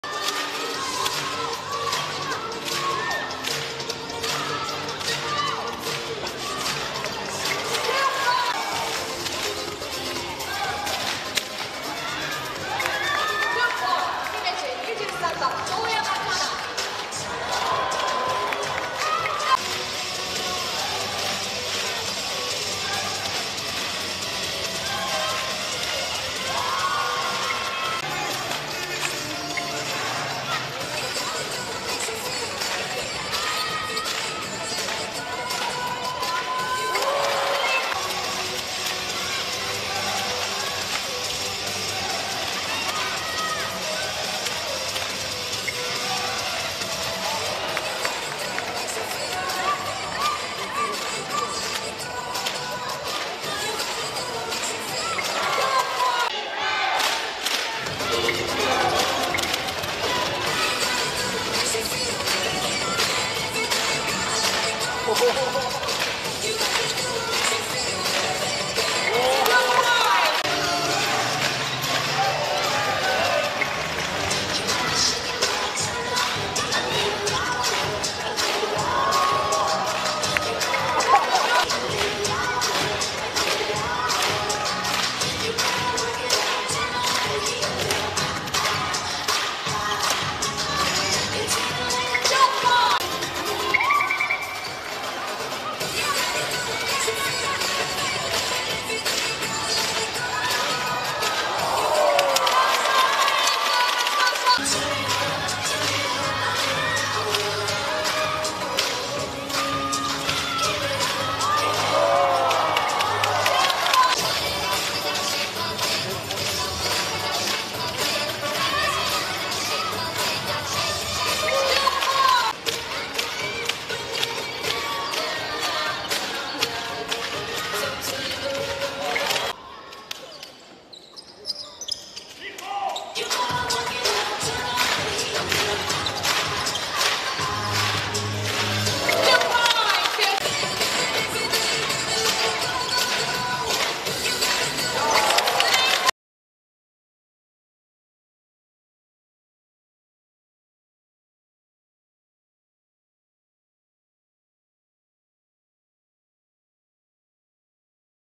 安間志織 Shiori Yasuma Highlights (vs 姫路イーグレッツ) 2023/10/14-10/15
トヨタアンテロープス - 姫路イーグレッツ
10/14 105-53
安間志織 4PTS 5REB 10AST 3STL 30:05
10/15 91-57
安間志織 10PTS 1REB 3AST 1STL 22:11